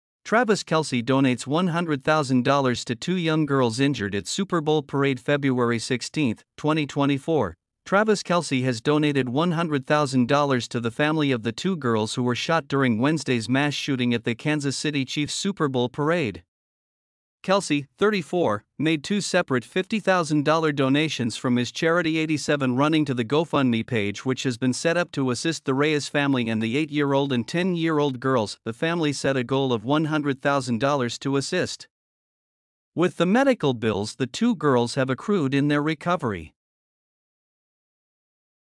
Travis Kelce has donated $100,000 to the family of the two girls who were shot during#traviskelce
video voice : ttsfree.
video information : Google
video/image present frome : Google

Travis Kelce has donated $100,000 to the family of the two girls who were shot during Wednesday’s mass shooting at the Kansas City Chiefs’ Super Bowl parade.
Kelce, 34, made two separate $50,000 donations from his charity Eighty-Seven & Running to the GoFundMe page — which has been set up to assist the Reyes family and the 8-year-old and 10-year-old girls.

@thesniffer2898 ‎@NatFL  ‎@Entertainment Tonight  ‎@The Celebrity Assets